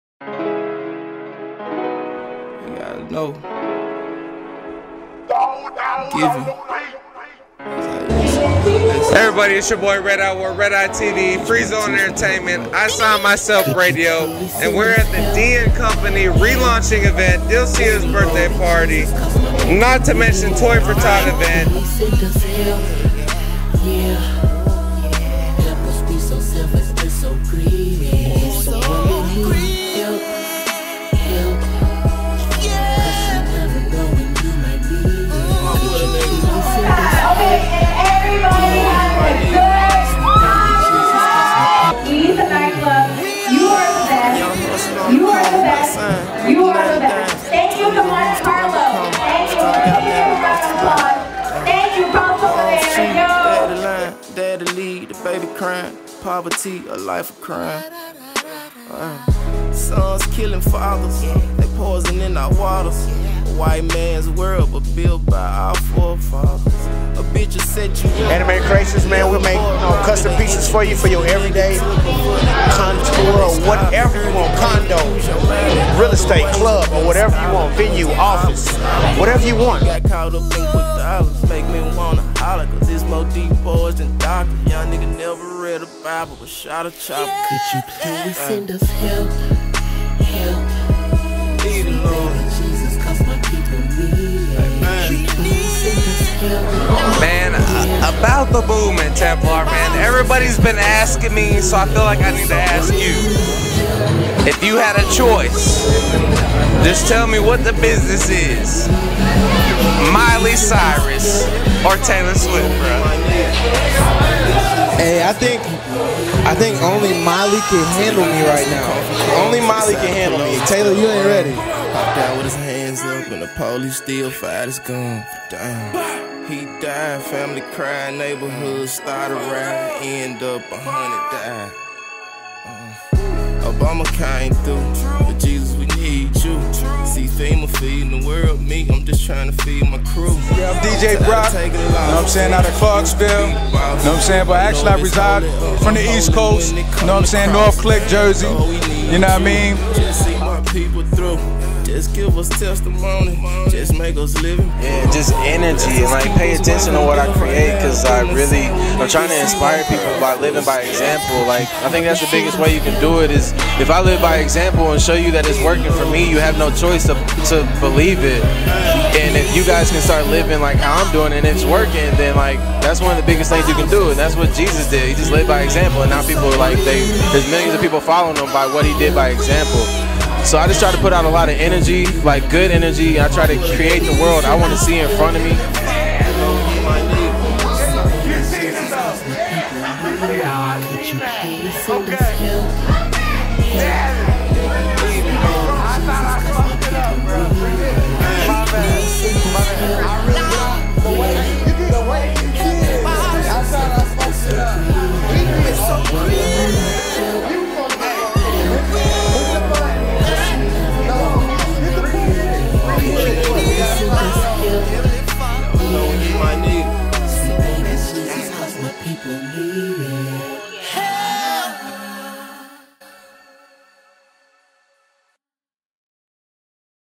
ToysforTot,DncompanyRecap
This video is about Toys for Tot,Dncompany Recap. Filmed at Ibiza nightclub in Nashville TN. Red Eye TV production.